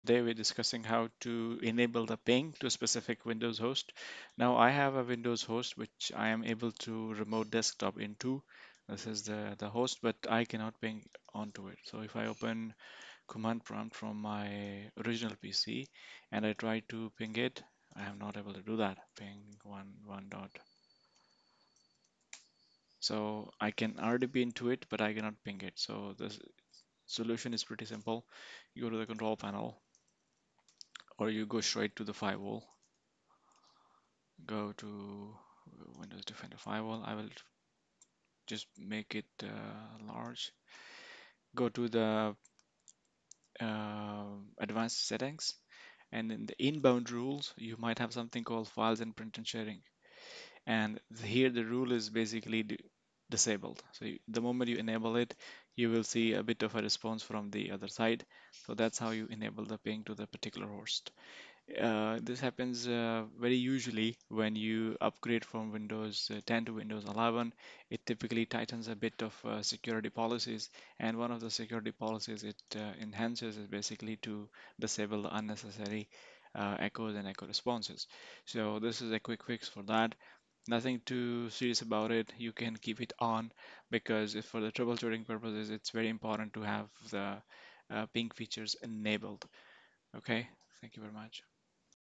How to enable ping on windows host | Enable ping on windows 10 11 server
Have you been struggling to enable ping on your windows 10 or 11 or server? If yes, then this video is the right one for you. In this video, I'll show you the easy steps to enable ping on individual computers and networks.